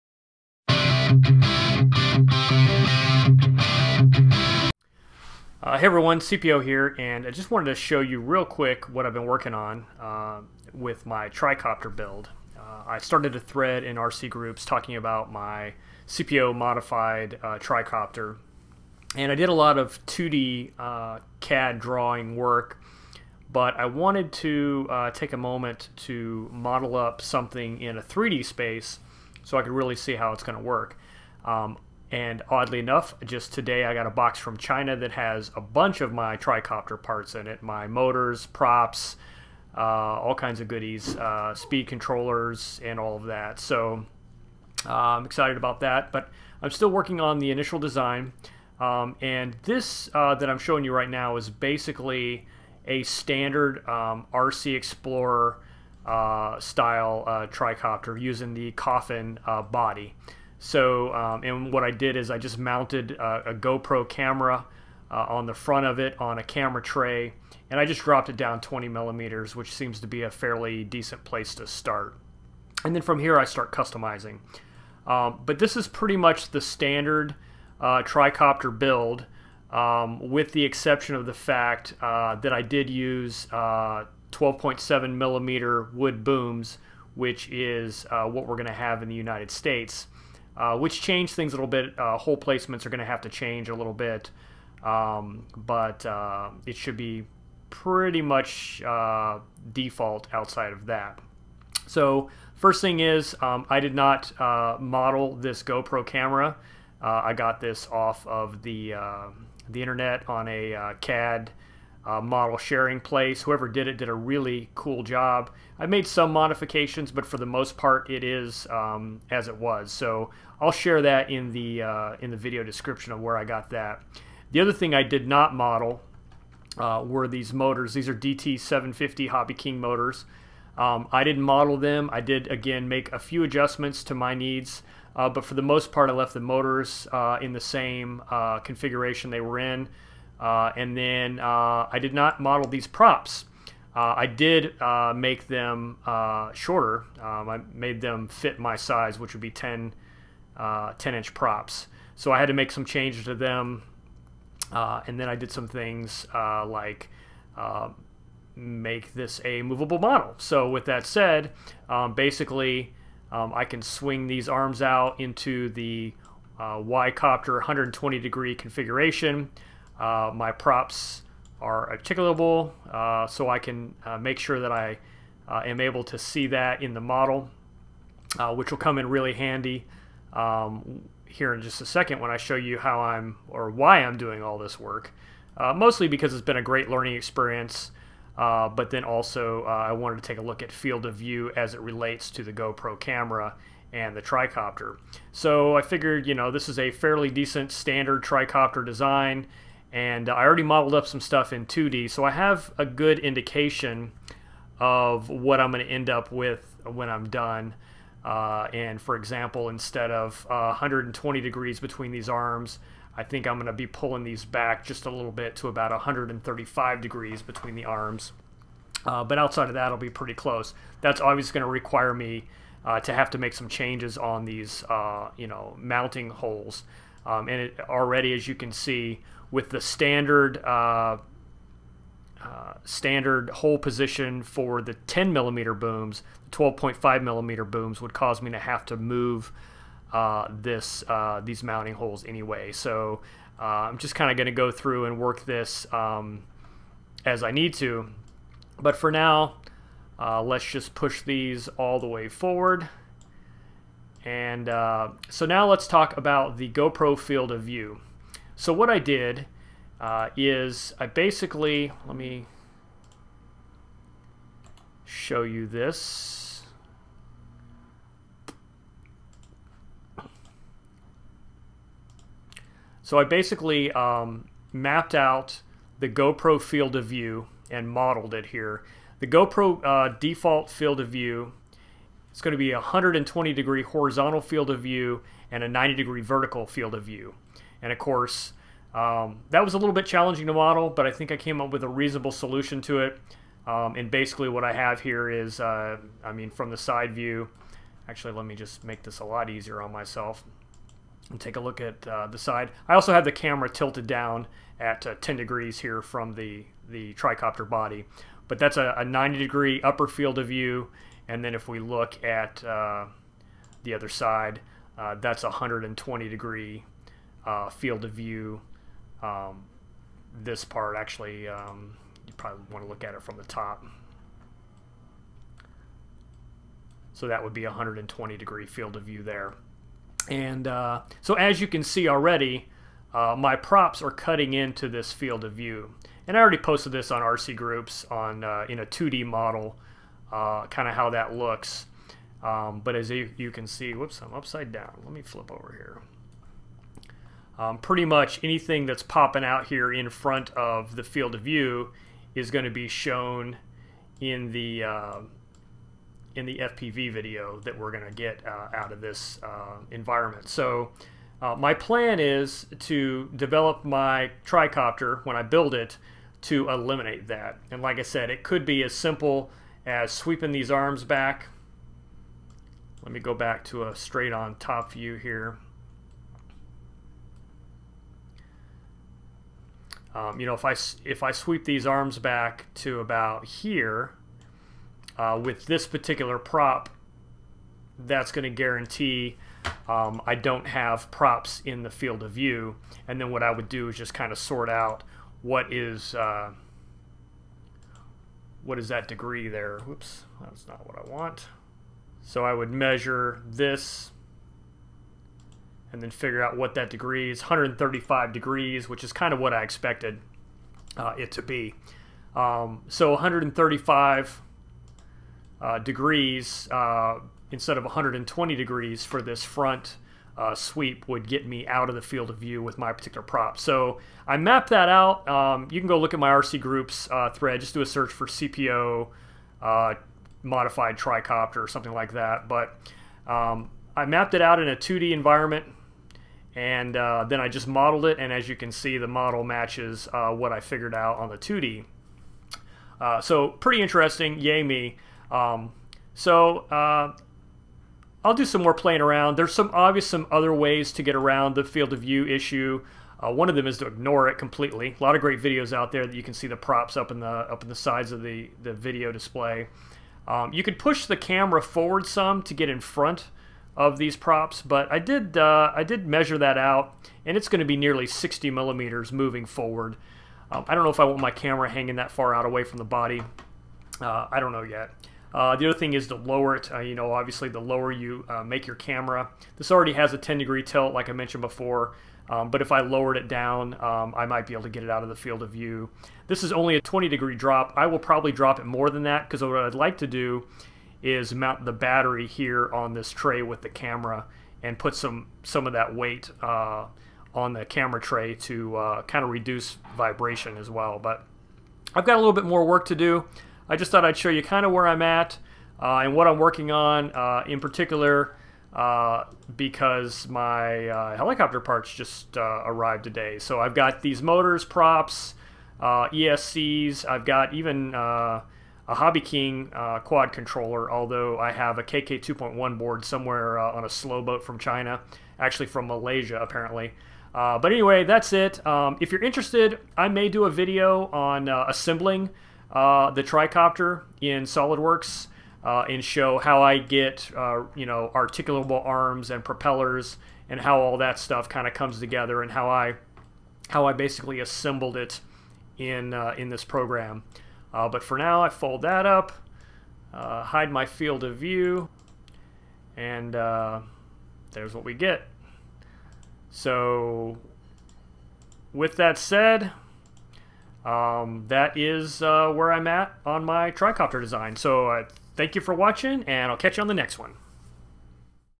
CPO Quick Look : Tricopter 3D Modeling to Design Around GoPro FOV
In this video, I show you the 3D modeling I have been working on with regard to my tricopter build process.  I decided to build the copter around the GoPro camera's standard field of view (120° x 90°) since that is what I plan on mounting to the front.  The standard rcexplorer.se design by David Windestål is a perfect platform to start with...so that's what I did.

I assembled the tricopter with 12.7mm booms at 400mm length with HexTronik DT450 motors and 10" props.  I used the coffin style body for simplicity (since I will be hand cutting my design out of plywood).

I modeled the GoPro field of view and then take a look at the model to see what I can do to keep the props out of the picture. It's been a great learning exercise!